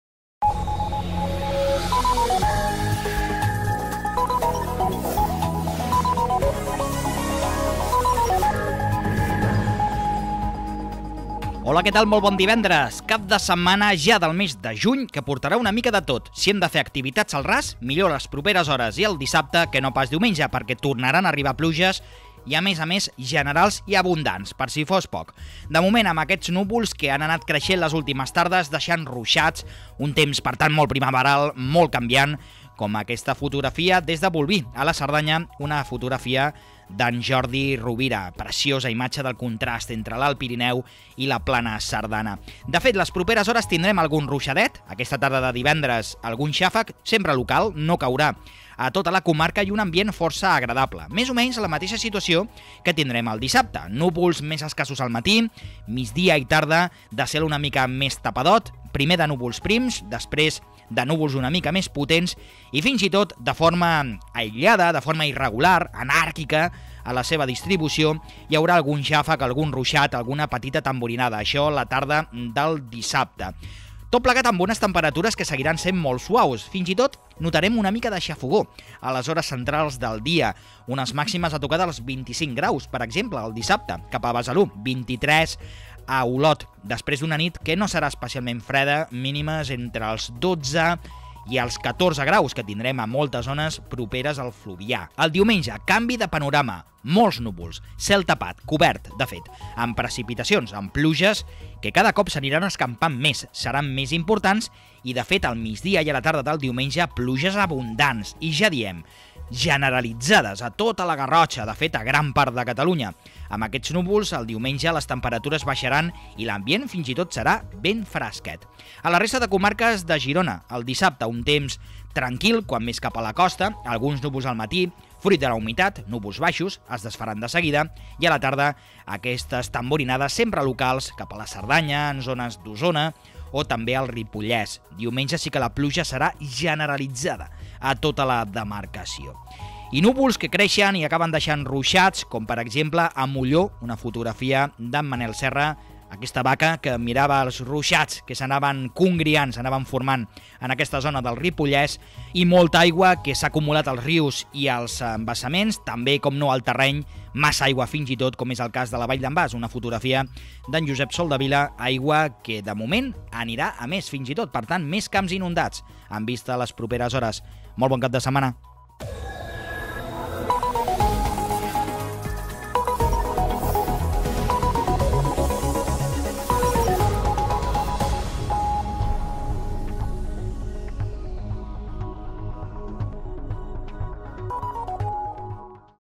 El temps divendres 1 juny 2018
Si heu de fer activitats al ras, millor dissabte